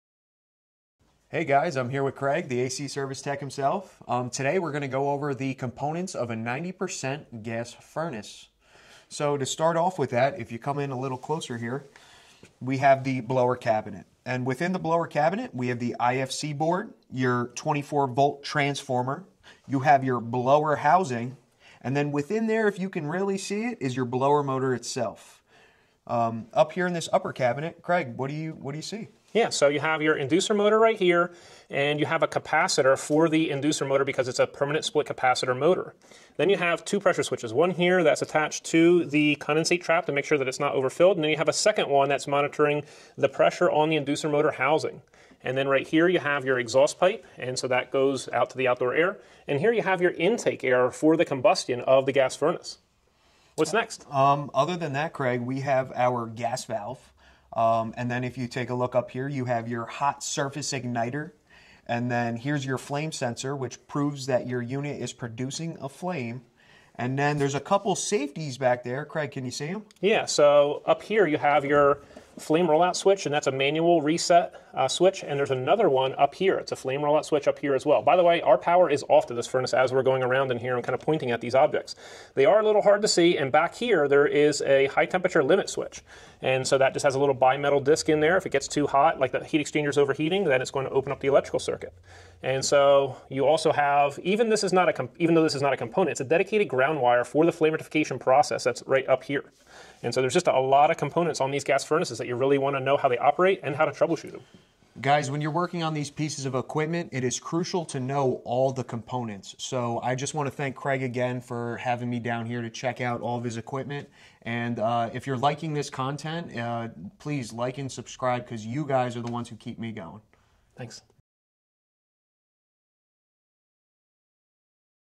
Inside a 90% Gas Furnace | Component Walkthrough for HVAC Techs
In this video, we open up a 90% efficient gas furnace and quickly point out the major components every HVAC tech should recognize — filmed alongside Craig from @acservicetechchannel and brought to you by Elite Service Group.

We keep it simple: identifying each part so viewers can get familiar with what they’re seeing inside a high-efficiency furnace.

🧰 In this walkthrough, we point out:

Inducer motor & pressure switch
Gas valve & ignitor
Control board & wiring
Blower motor & condensate trap
Vent piping and airflow path
Whether you're new to the field or helping someone learn, this visual breakdown gives you a fast, no-fluff look at what’s behind the panel of a 90% gas furnace.

🎥 Filmed in collaboration with @acservicetechchannel
🔧 Powered by Elite Service Group